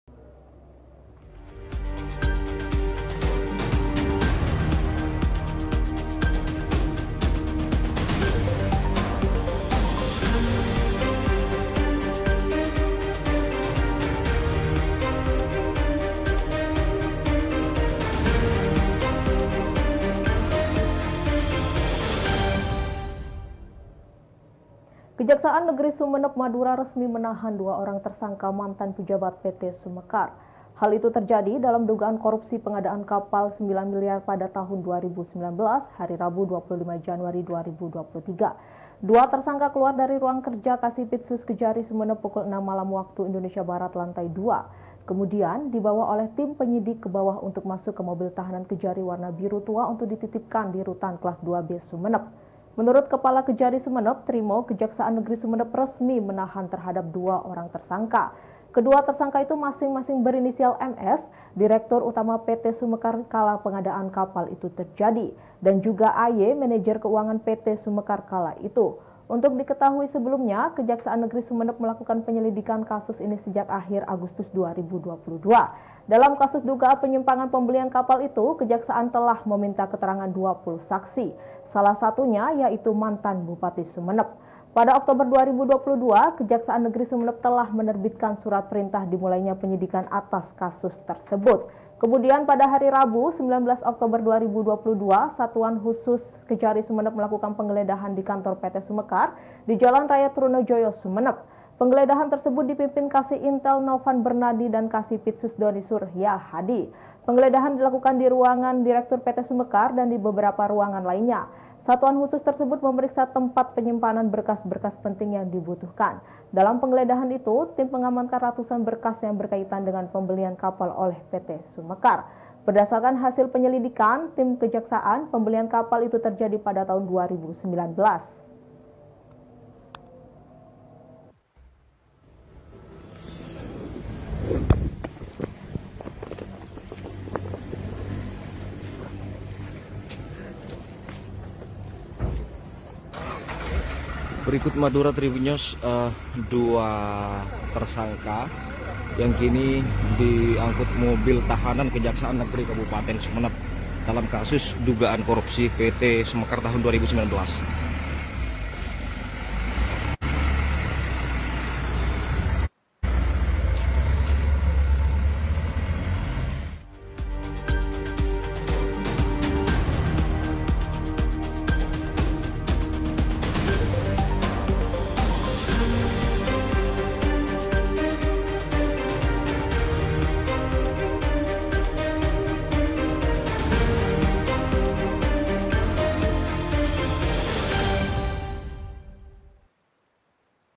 2 Mantan Pejabat PT. Sumekar Resmi Ditahan Kejari Sumenep, Kasus Korupsi Pengadaan Kapal Rp 9 Miliar
SUMENEP - Kejaksaan Negeri (Kejari) Sumenep Madura resmi menahan 2 orang tersangka mantan pejabat PT. Sumekar dalam dugaan korupsi pengadaan kapal Rp 9 Miliar pada Tahun 2019 hari Rabu (25/1/2023) pukul 18.25 WIB.

Pantauan TribunMadura.com di kantor Kejari Sumenep, 2 tersangka keluar dari ruang kerja Kasi Pidsus Kejari Sumenep pukul 18.00 WIB lantai dua.

Kemudian dibawa oleh tim penyidik ke bawah untuk masuk ke mobil tahanan Kejari warna biru tua untuk dititip di Rutan Klas IIB Sumenep.